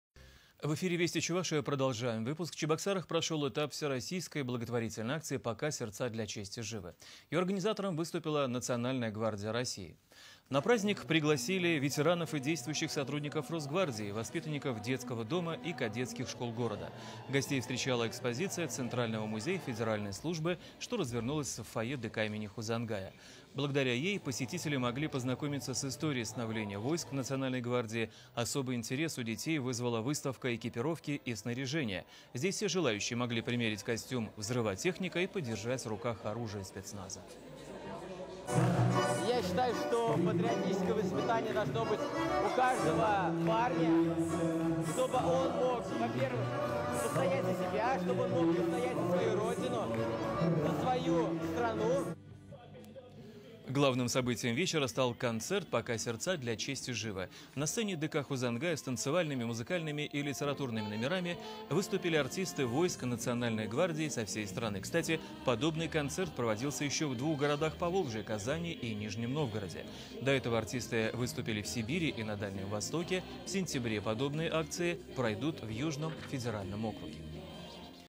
В Чебоксарах прошла патриотическая акция «Пока сердца для чести живы»
В Чебоксарах прошел этап Всероссийской благотворительной акции «Пока сердца для чести живы».